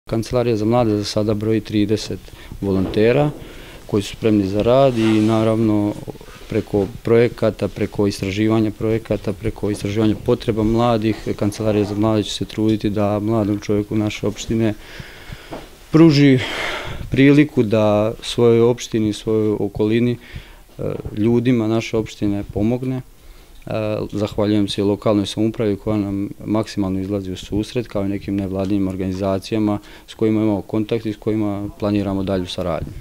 Kancelarija za mlade u Šidu nastavlja sa radom
Kancelarija za mlade u Šidu nastavlja sa radom.  Predstavnici su danas posetili  srednju tehničku školu “Nikola Tesla”, kako bi učenike koji su u đačkom parlamentu  pozvali da  se pridruže  volonterskim akcijama I predstave svoje ideje.